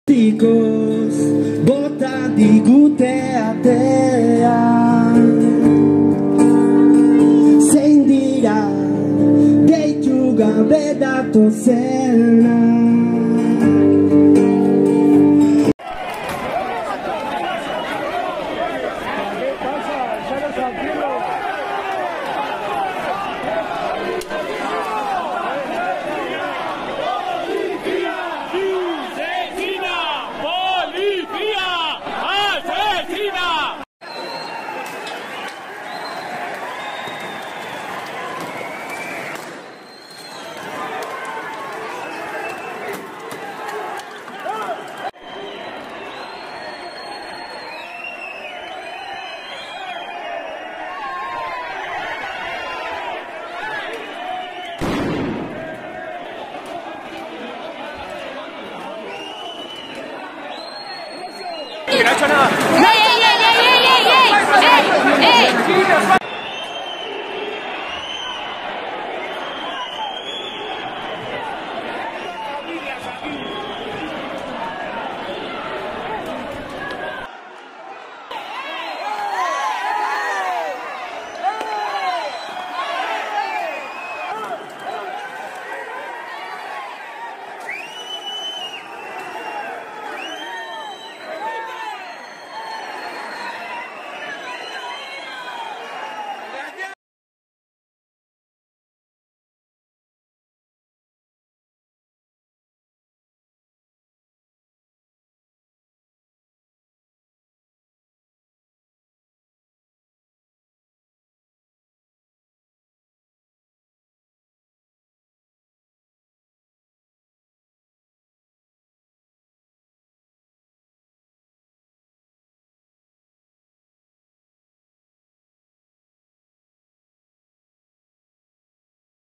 Martxoaren 3ko manifestazioa
| Istiluak izan dira 1976ko martxoaren 3ko
sarraskia oroitzeko manifestazioan

2025erako 3.000 babesle berri behar ditugu, iragana eta geroa orainaldian kontatzeko.